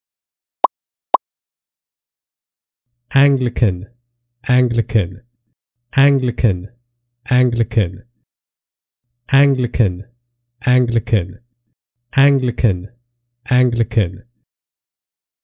How to Pronounce Anglican
In this video we'll show you how to correctly pronounce:  Anglican.